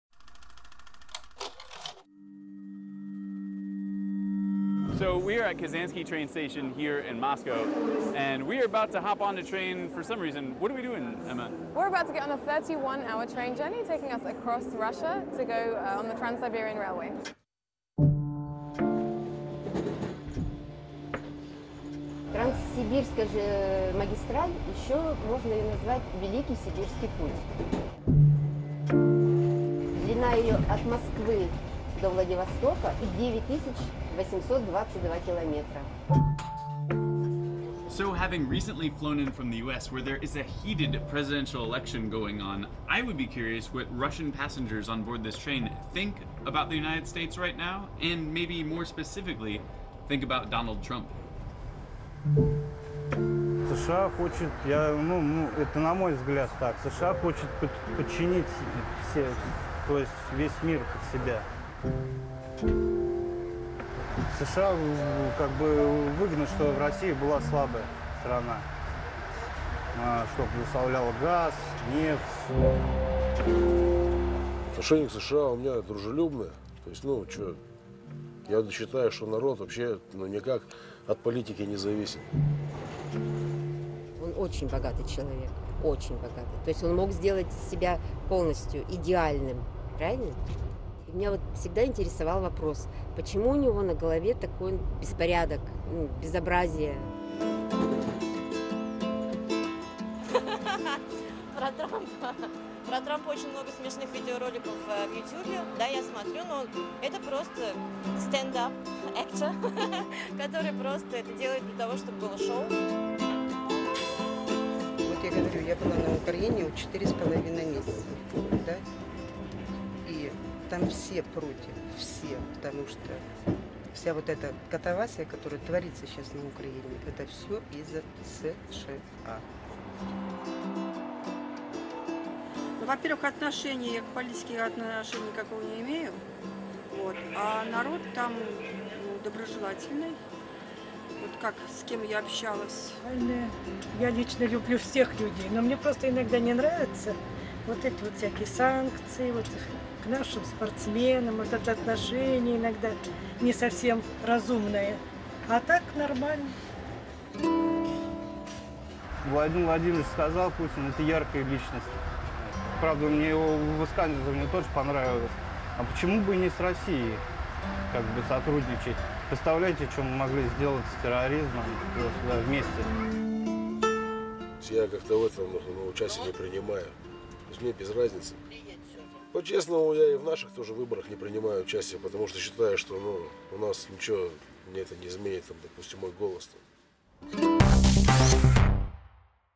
BBC Pop Up в России: пассажиры поезда о США и Трампе
BBC Pop Up, передвижное бюро Би-би-си, отправилось в Россию, чтобы снять несколько коротких документальных фильмов для BBC World News, сайта BBC News и Русской службы Би-би-си.

На этот раз наши корреспонденты отправились в путешествие на поезде через всю Россию по Транссибирской железной дороге.

Они спросили у пассажиров поезда, что они думают о президентской предвыборной кампании в США, а точнее - о Дональде Трампе.